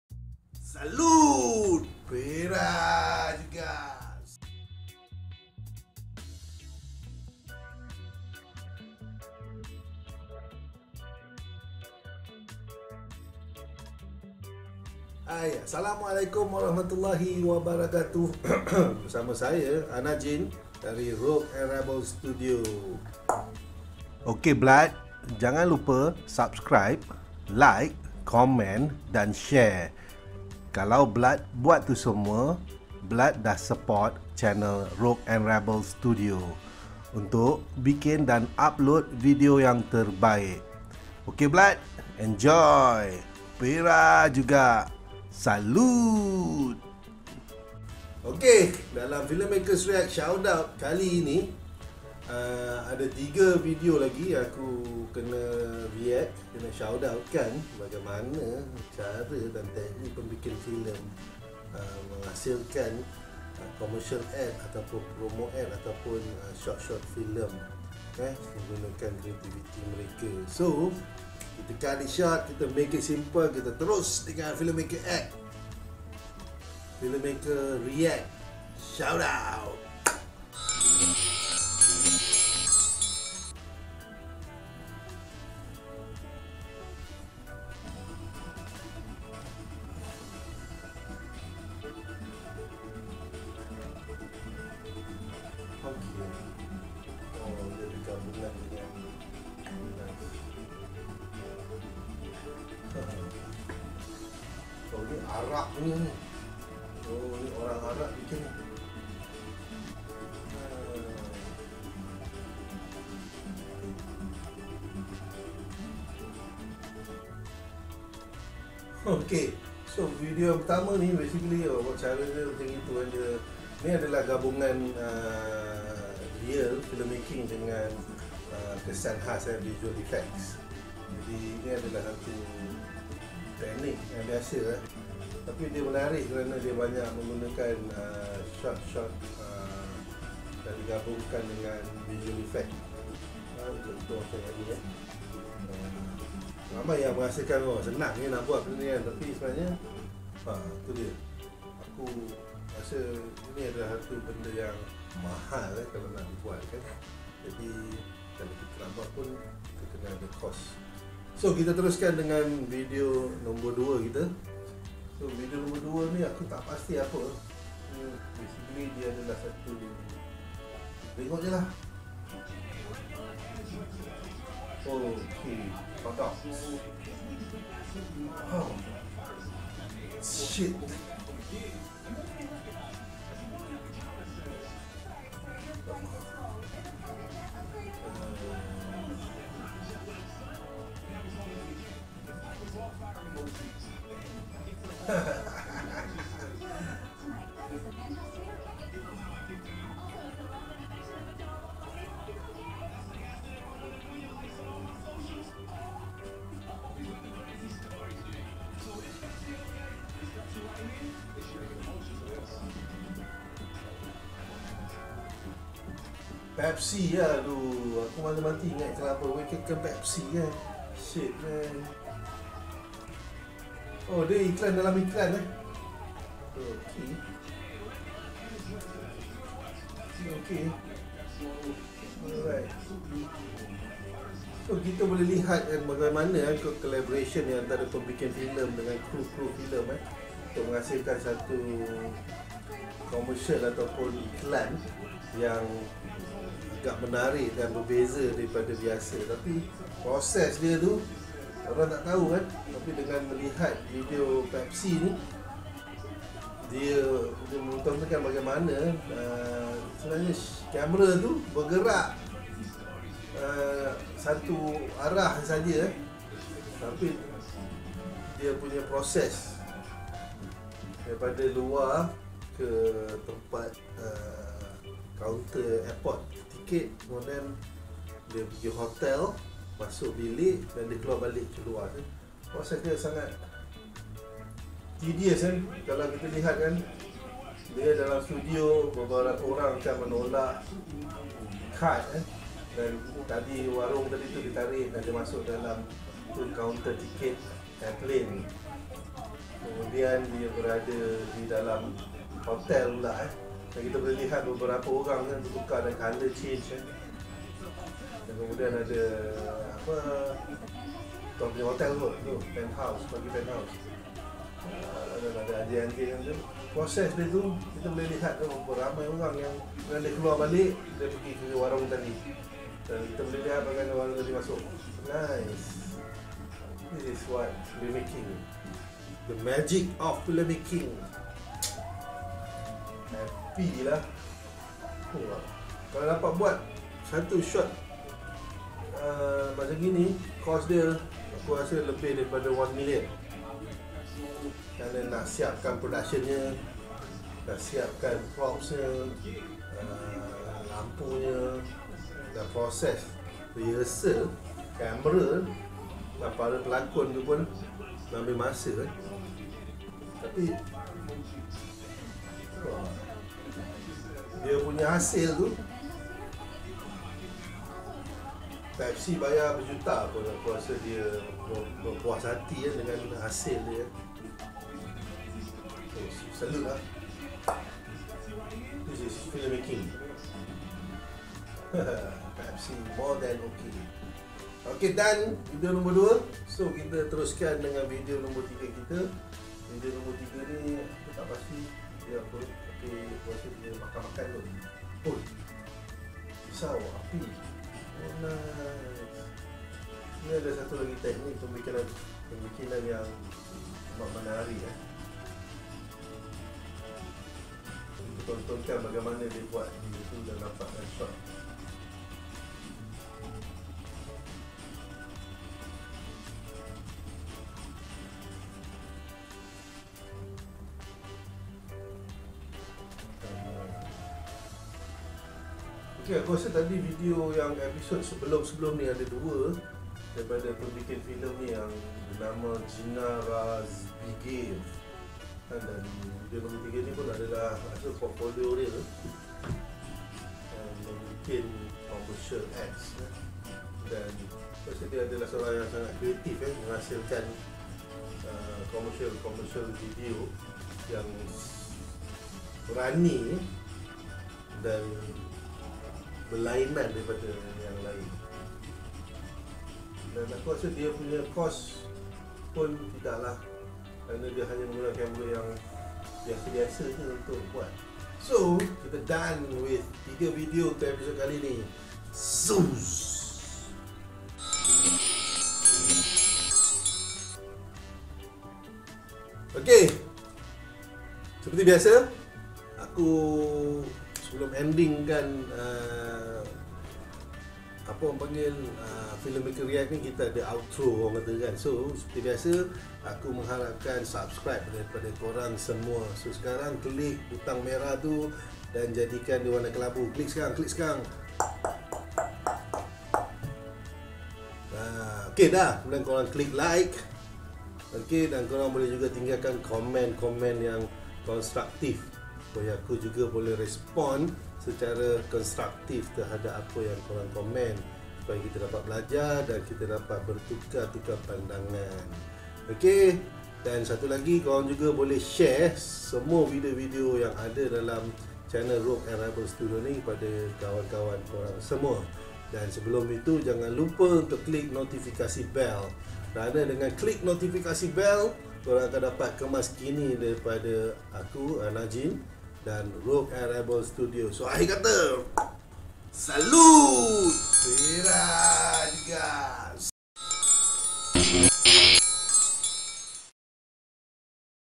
Filmmaker Shout Out #04 - FILMMAKING MAGIC (2019)
Anajin Filmmaker Shout Out adalah satu siri baru dimana Anajin akan memberi reaksi terhadap teknik bikin filem yang menghasilkan visual untuk penghasilan produk iklan komersil oleh pembikin filem seluruh dunia.

Sebarang cadangan dan video magika bikin filem boleh ditulis di ruangan komen.